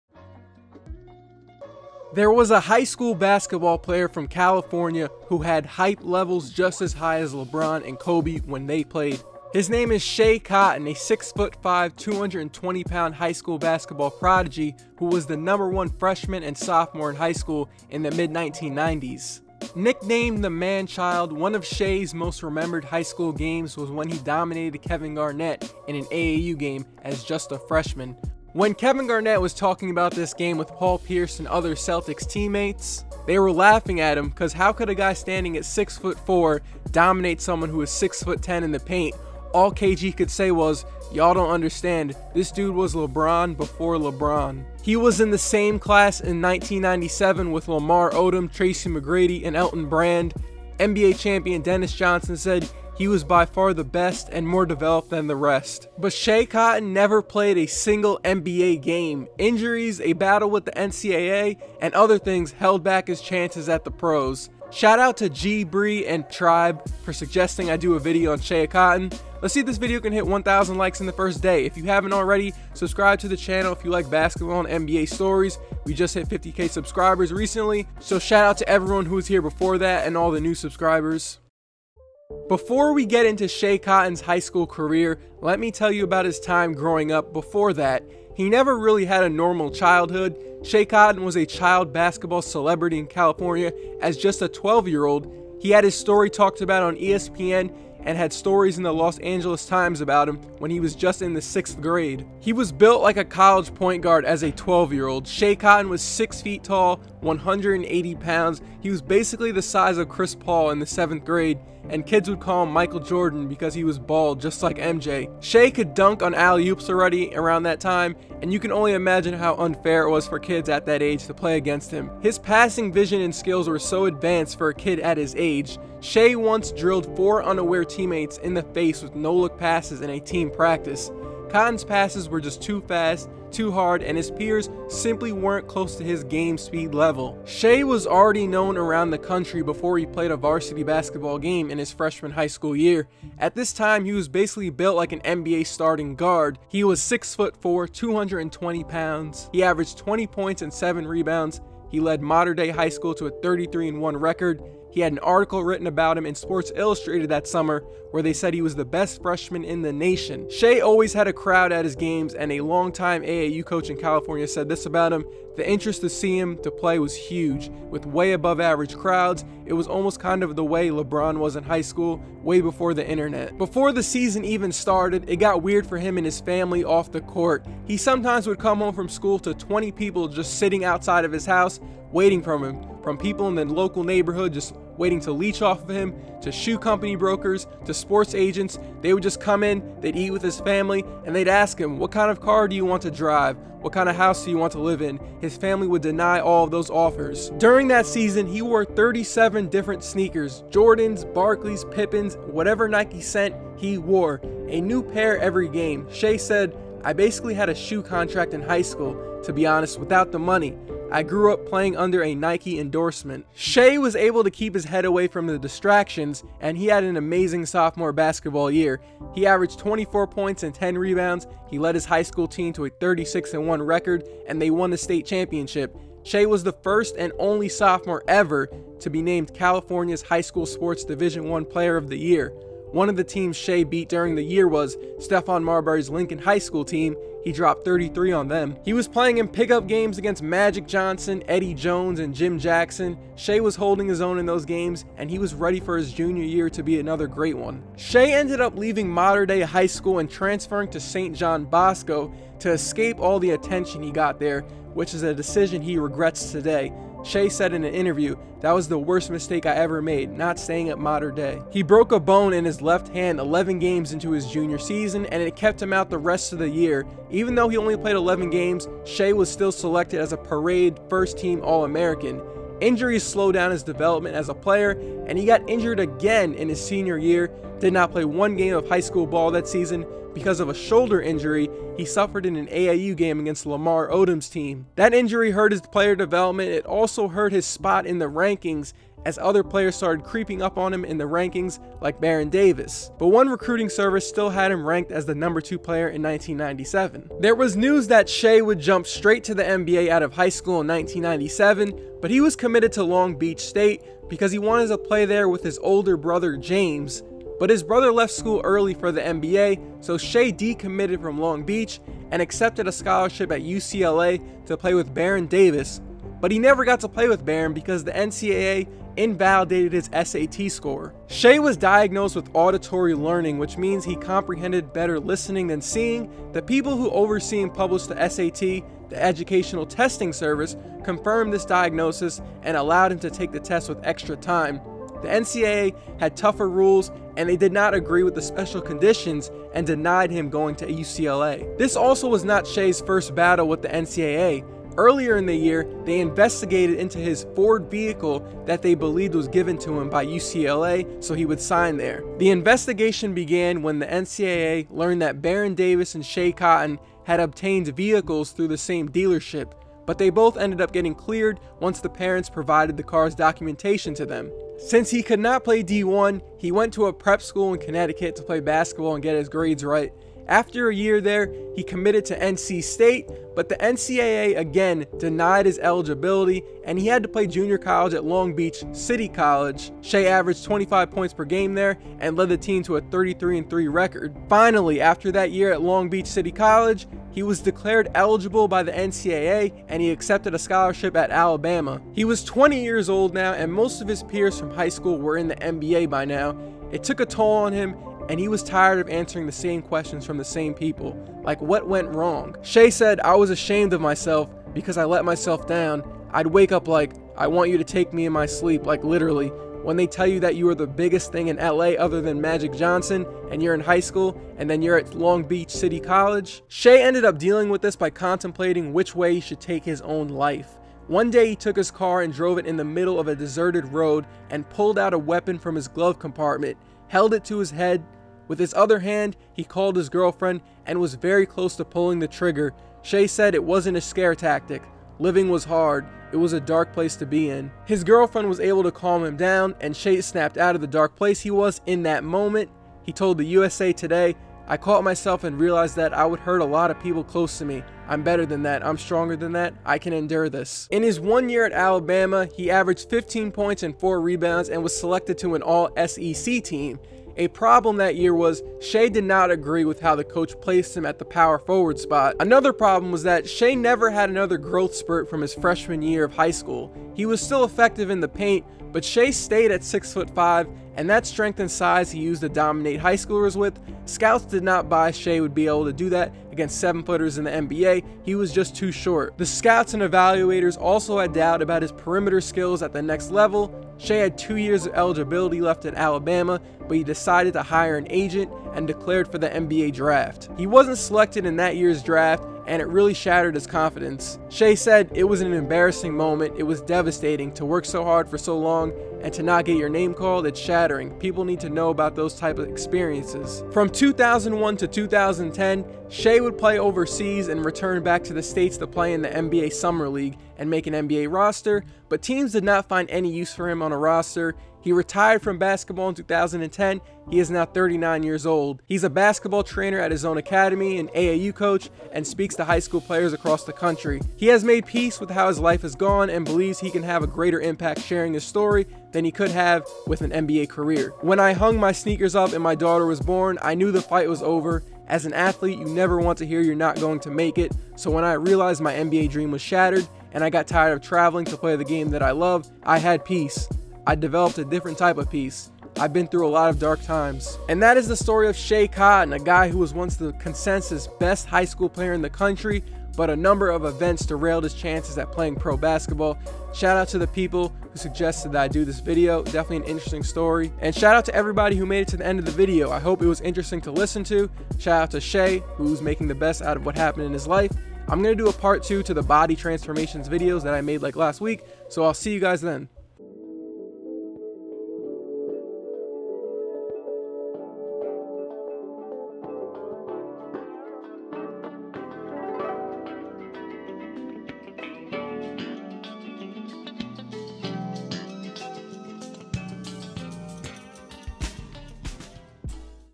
Meet SCHEA COTTON: He Was LeBron before LeBron in High School
Schea Cotton was considered LeBron James before LeBron James in high school and had a Nike Shoe Deal As a Sophomore! What Happened?

There was a high school basketball player from California who had hype levels just as high as LeBron James and Kobe Bryant.

His name is Schea Cotton, A 6 foot 5, 210 pound high school basketball prodigy who was the number one freshmen, sophomore and junior in high school in the mid 1990’s at Mater Dei. But he never played a game in the NBA!

sources: sources